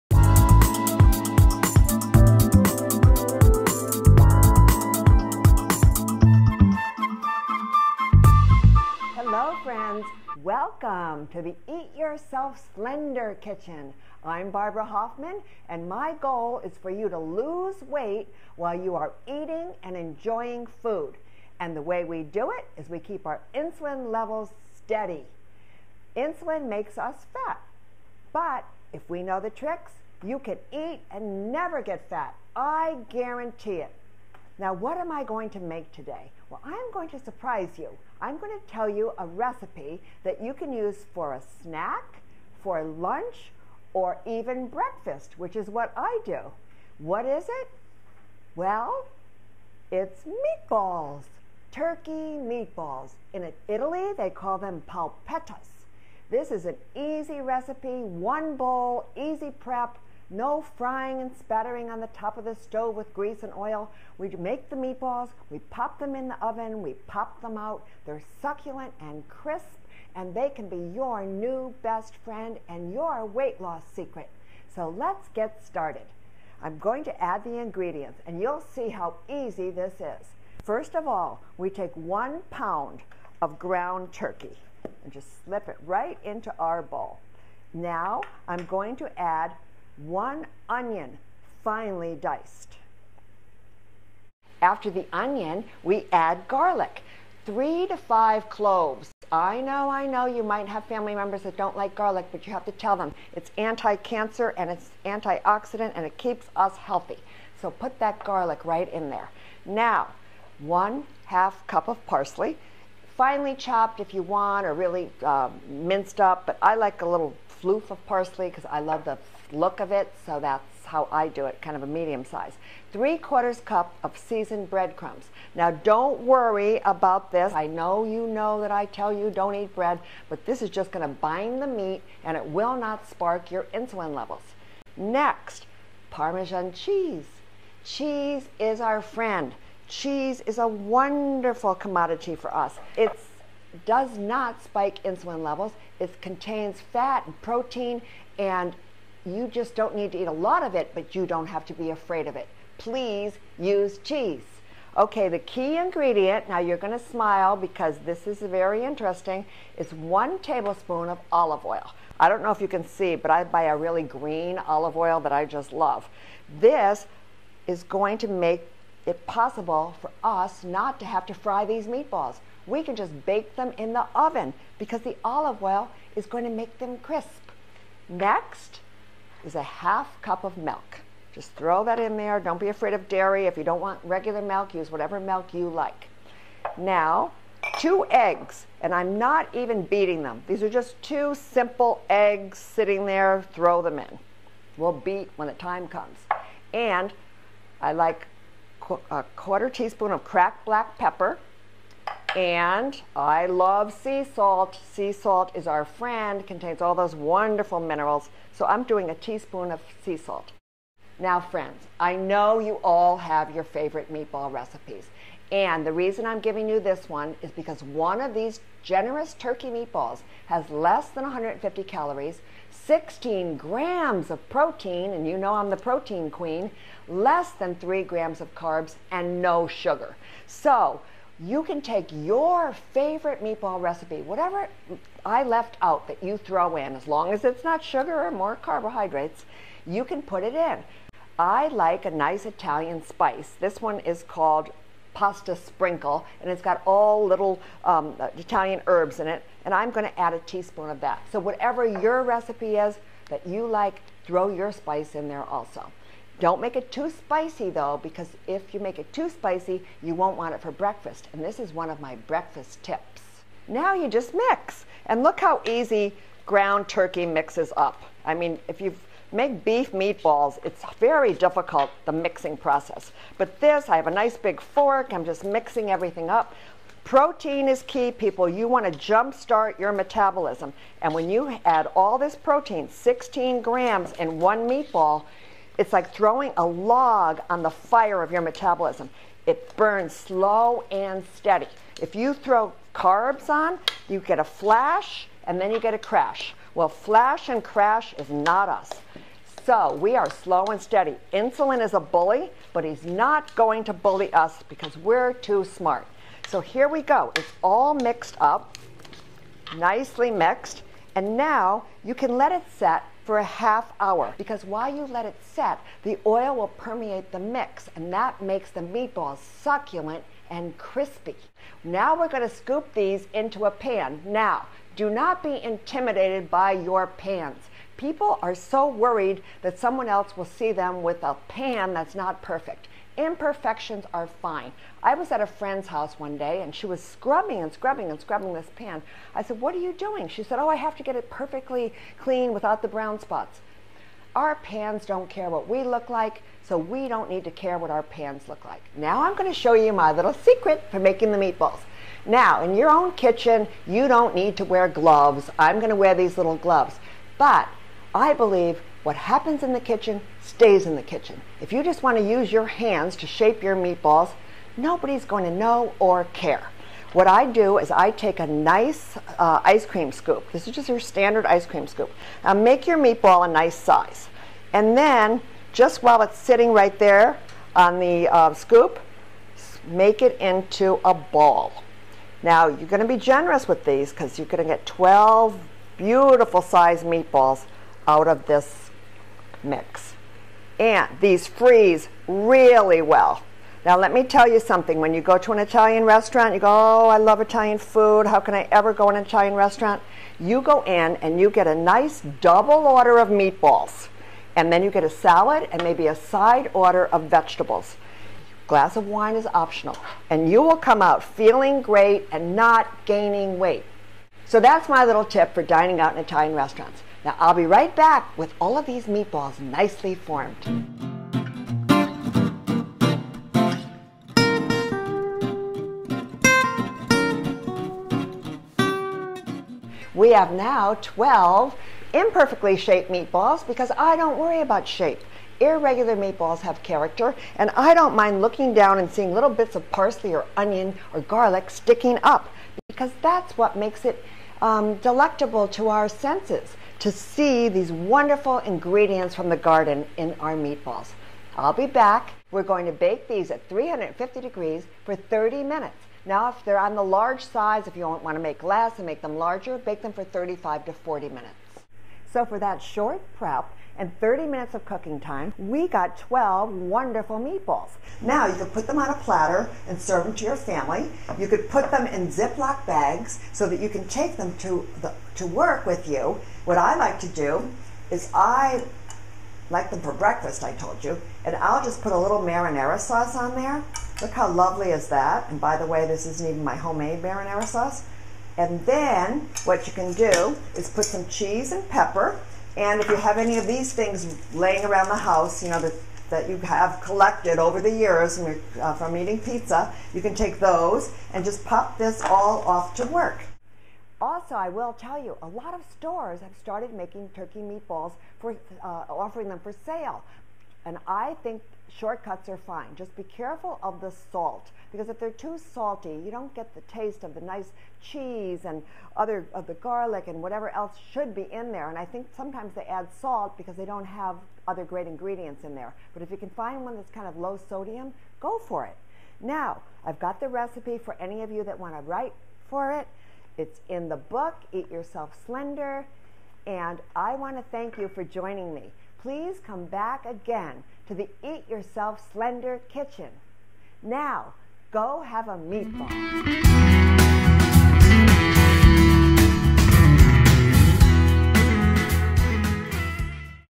Barbara's Unbelievably Delish Turkey Meatballs
In this episode of the Eat Yourself Slender Kitchen Series, I will show you how to make one of my favorite recipes - turkey meatballs! Good for breakfast, lunch, and dinner, these meatballs are a great source of protein with 16 grams packed in each meatball!

Ingredients:
• 1 pound ground turkey
• 3 -- 5 garlic cloves, minced
• ½ cups finely chopped parsley
• 2 eggs
• ½ cups milk
• ½ cup grated cheese
• ¾ cups Italian bread crumbs
• 1 TBS olive oil
• 1 onion, minced finely
• 1 Tsp. sea salt
• ½ Tsp. cracked black pepper

Directions:
Place all ingredients in large bowl and mix thoroughly. Let stand half an hour.

Note: You can add any other seasonings of your choice. For example, 1 Tsp. Italian spice.
Shape into medium size meatballs.
Makes about 12 meat balls.

Place on cookie sheet and bake at 350 degrees for 30-35 minutes until meatballs look slightly browned.
My favorite snack! These freeze really well!

Don't worry! -- That ¾ cup of bread crumbs "binds" the meat balls and won't spike your insulin.

Barbara is the author of Eat Yourself Slender, a book that turns dieting on its head!